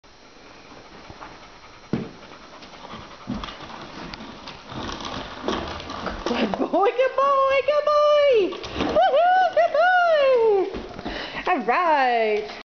Trooper skateboarding
Trooper the Vizsla skateboarding.